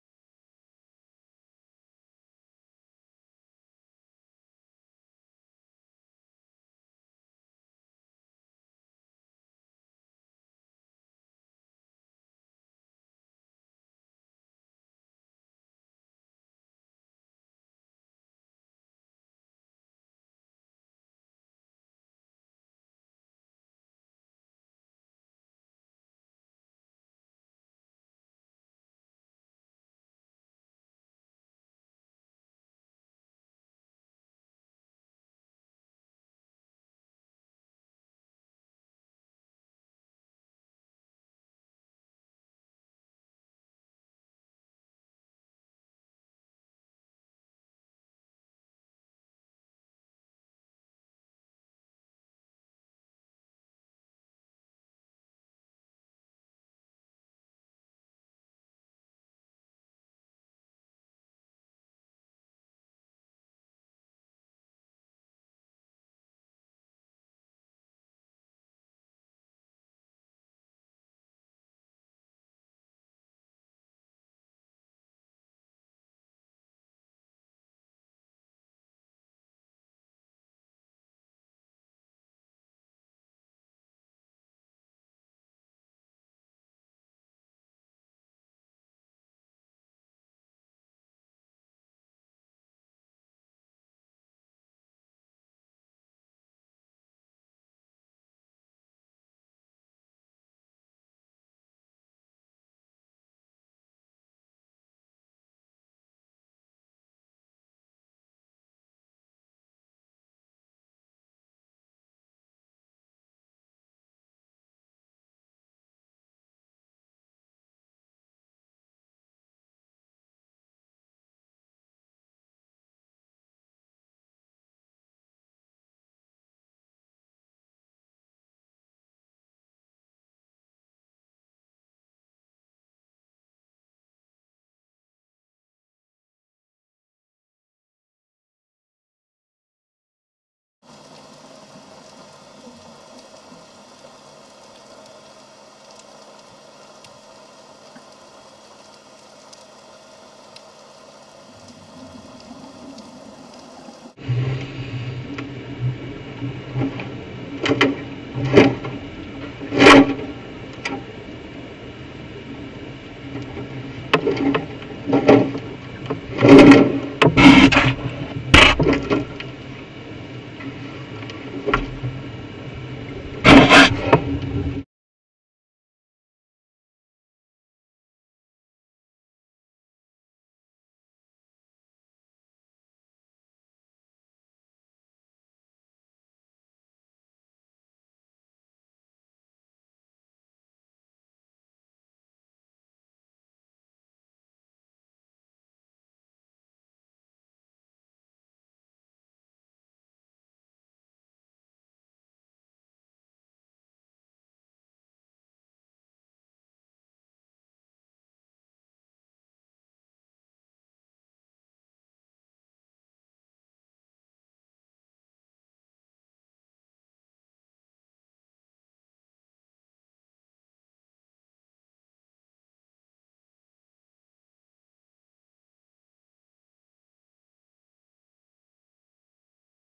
Freediving tiger sharks / Apnée avec des requins tigre!
Freediving among tiger sharks off the Durban coast. Video taken while freediving. No cage diving used!